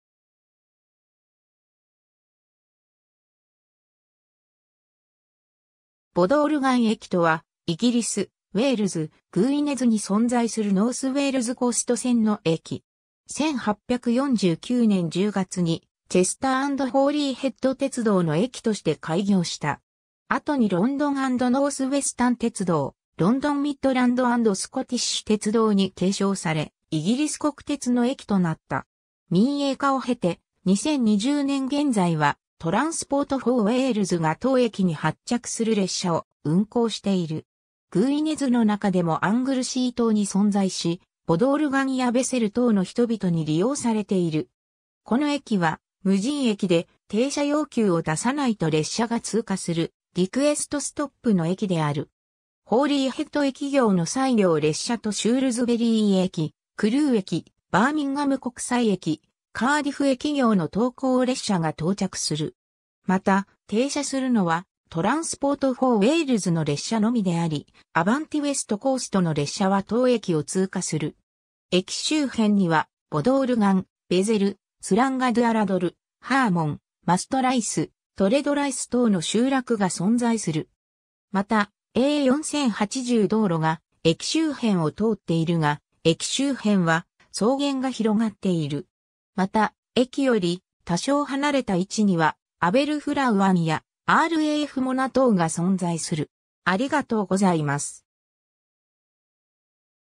ボドオルガン駅
ボドオルガン駅（英語:Bodorgan Station）とは、イギリス、ウェールズ、グウィネズに存在するノース・ウェールズ・コースト線の駅。
 1849年10月にチェスター・アンド・ホーリーヘッド鉄道の駅として開業した。
後にロンドン・アンド・ノース・ウェスタン鉄道、ロンドン・ミッドランド・アンド・スコティッシュ鉄道に継承されイギリス国鉄の駅となった。
民営化を経て、2020年現在はトランスポート・フォー・ウェールズが当駅に発着する列車を運行している。
 グウィネズの中でもアングルシー島に存在し、ボドオルガンやベセル等の人々に利用されている。
この駅は無人駅で、停車要求を出さないと列車が通過するリクエスト・ストップの駅である。
ホーリーヘッド駅行の西行列車とシュールズベリー駅、クルー駅、バーミンガム国際駅、カーディフ駅行の東行列車が到着する。
また、停車するのはトランスポート・フォー・ウェールズの列車のみであり、アヴァンティ・ウェスト・コーストの列車は当駅を通過する。
 駅周辺にはボドオルガン、ベゼル、スランガドゥアラドル、ハーモン、マストライス、トレドライス等の集落が存在する。
また、A4080道路が駅周辺を通っているが、駅周辺は草原が広がっている。
 また、駅より多少離れた位置にはアベルフラウ湾やRAFモナ等が存在する。